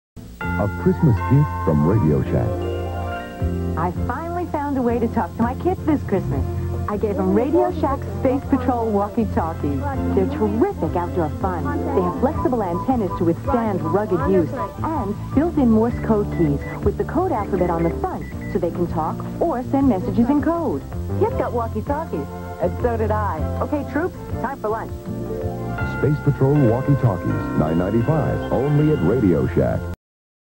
1983 Radio Shack Christmas commercial
This Radio Shack Christmas ad features their walkie-talkies...and if you were a child of the 80's as I was, you remember wandering around your (or your friend's) yard and working on secret government missions!